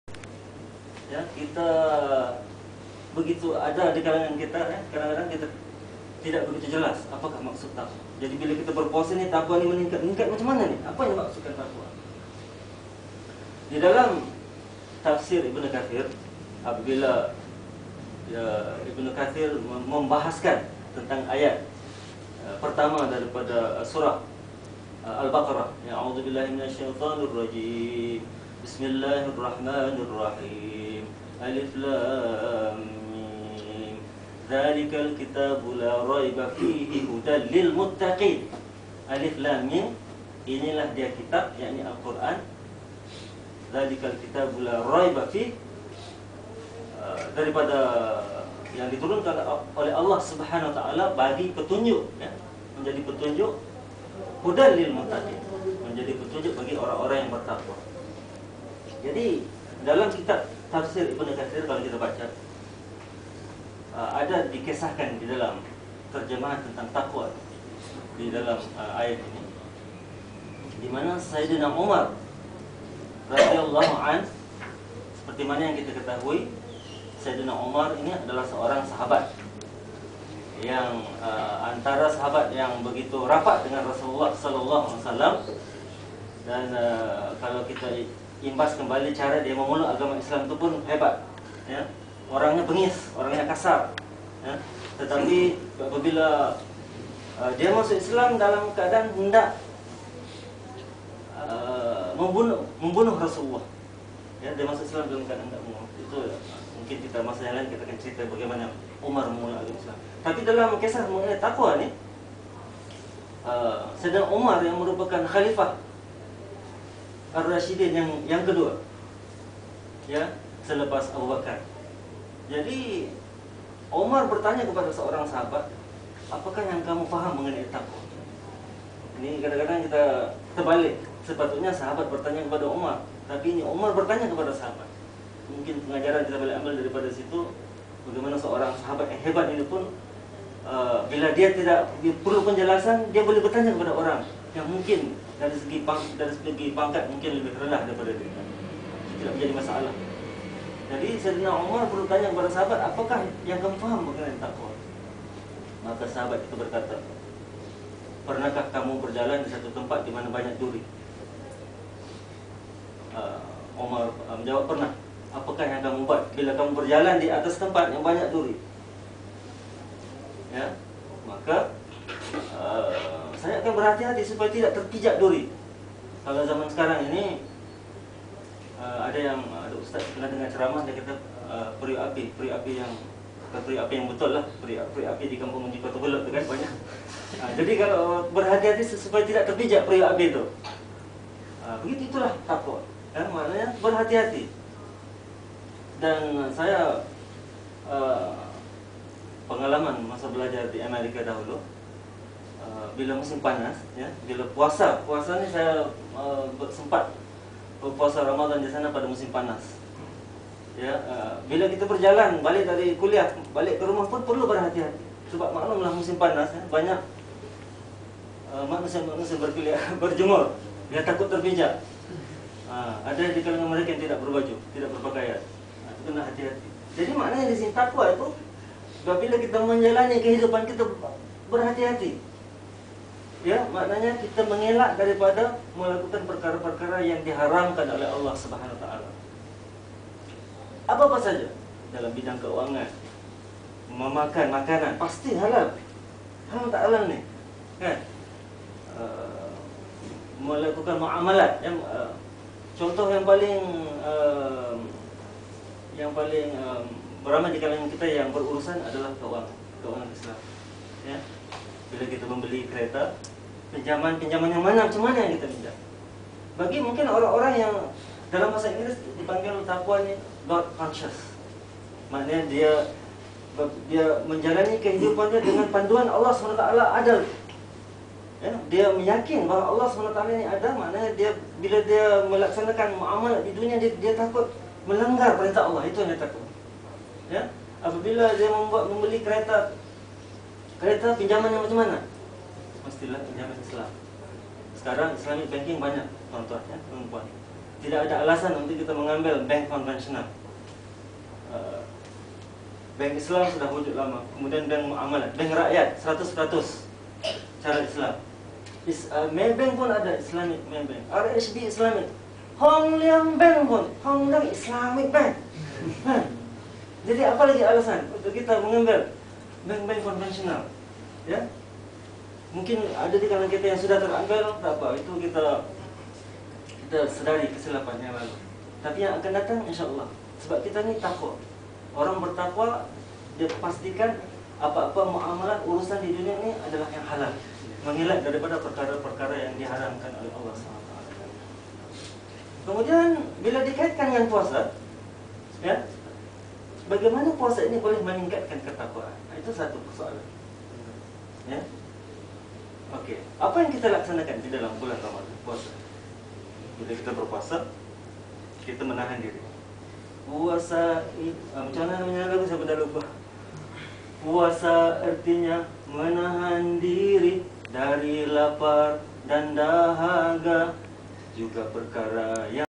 Takzirah Maghrib malam 7 Ramadan - Ustaz Marsus, Imperial Boutec Hotel
Tazkirah yang disampaikan oleh Ustaz Marsus pada majlis iftar di Imperial Boutec Hotel